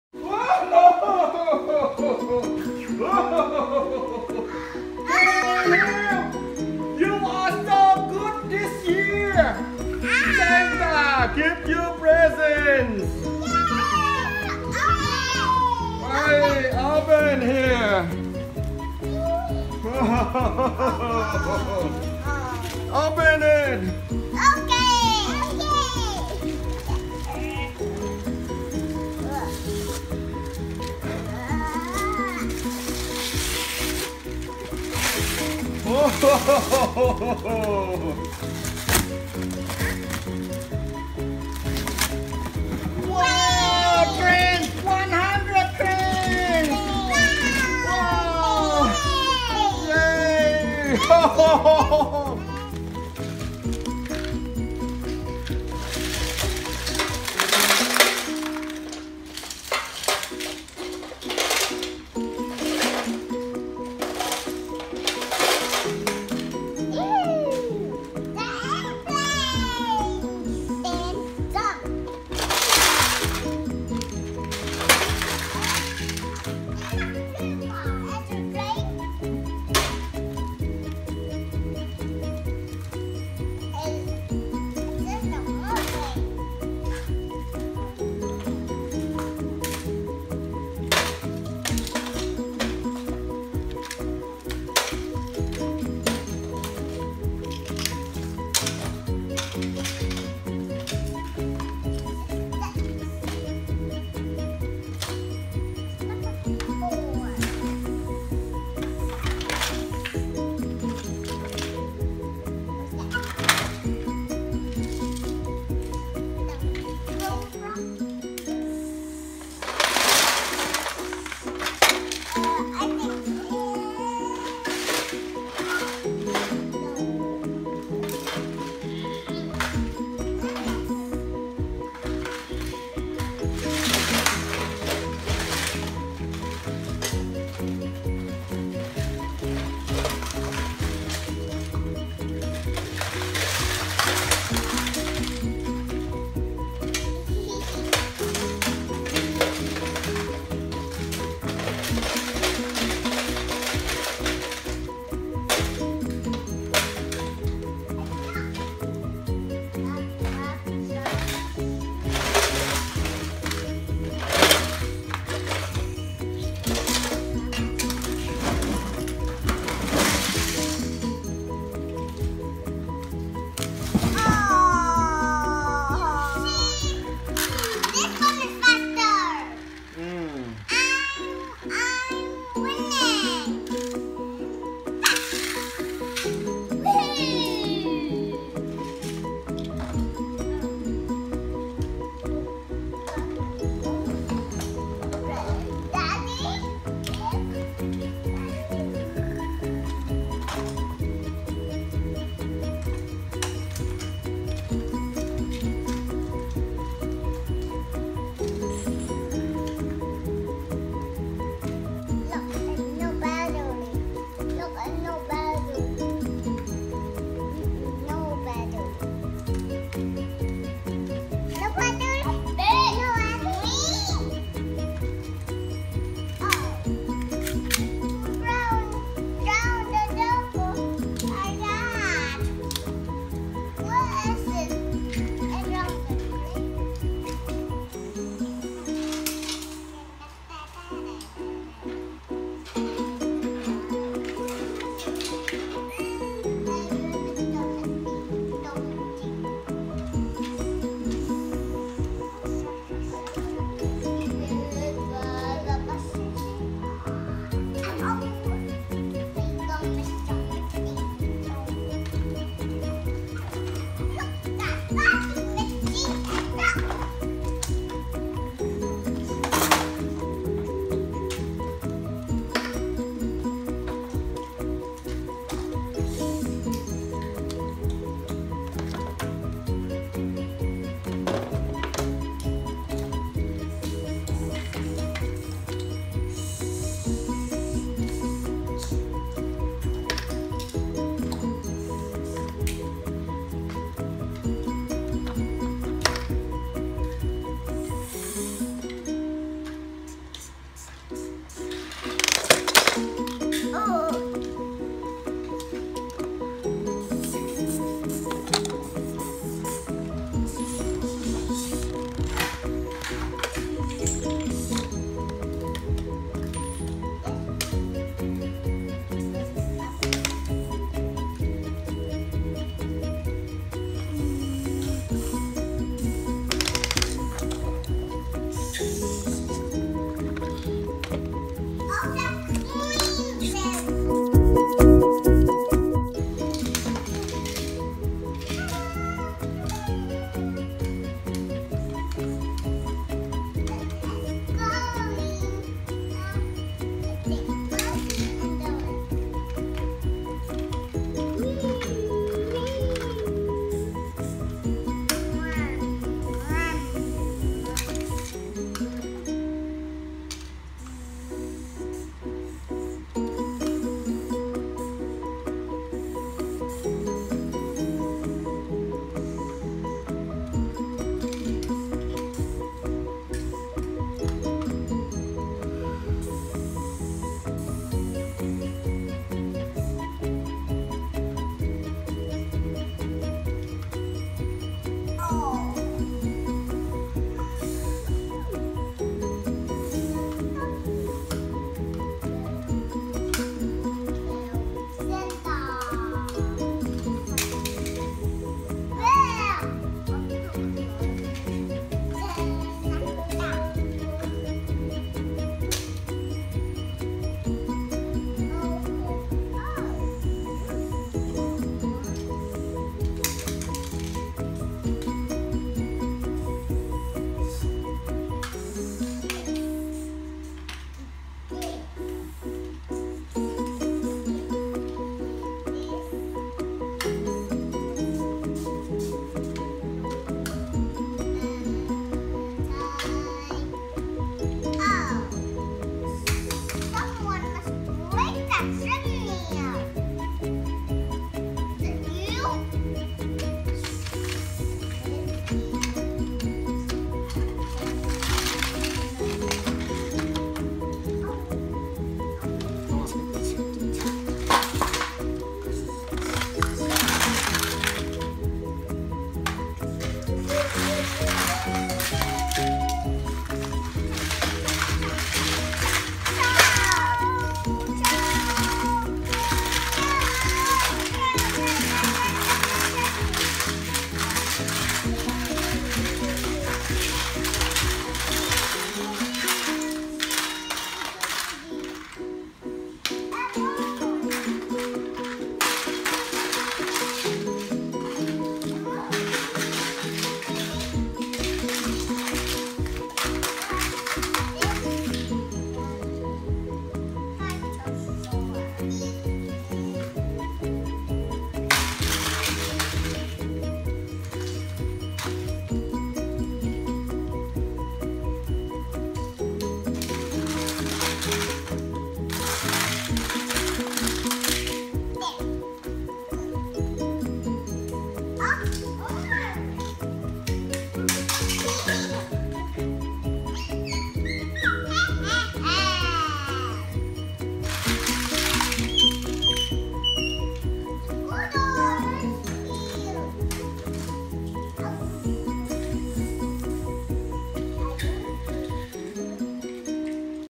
Christmas Day Kids Pretend Santa Claus ★ Santa Claus's Amazing Gifts For My Children
Santa came to give presents.  Ken and Hugh got 100 Thomas the Train toys.  They love Thomas the Train

Hi I'm Hugh. I 💖  Ride-on Cars 🚗  Disney Cars 🚗 Thomas and Friends Playdoh  🚆 Playdoh & fun 🎉 Pls vi
sit us at: 💥